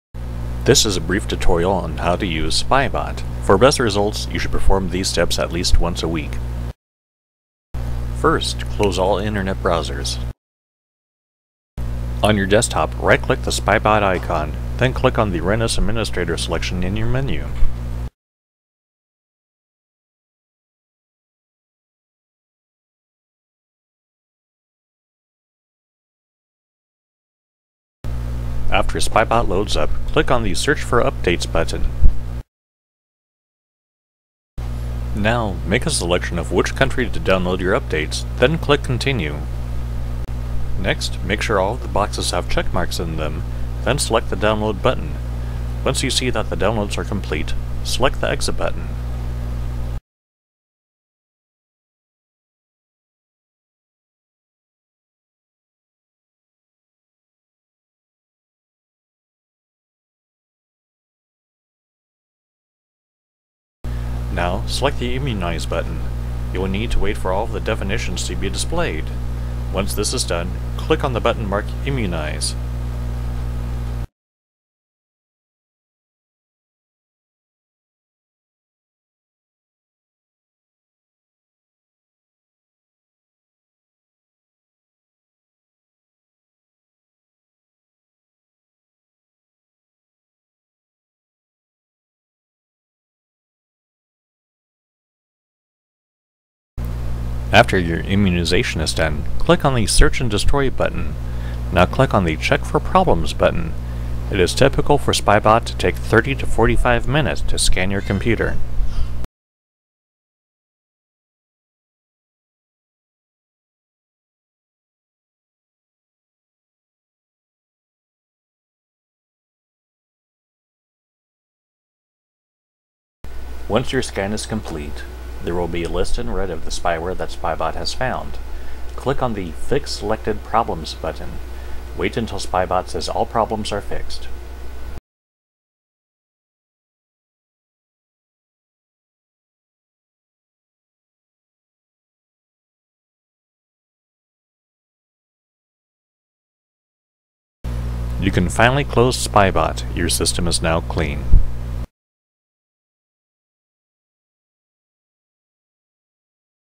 How to Use Spybot Search & Destroy
How to use Spybot Search & Destroy.  You should do this routine at least once a week for optimal  results.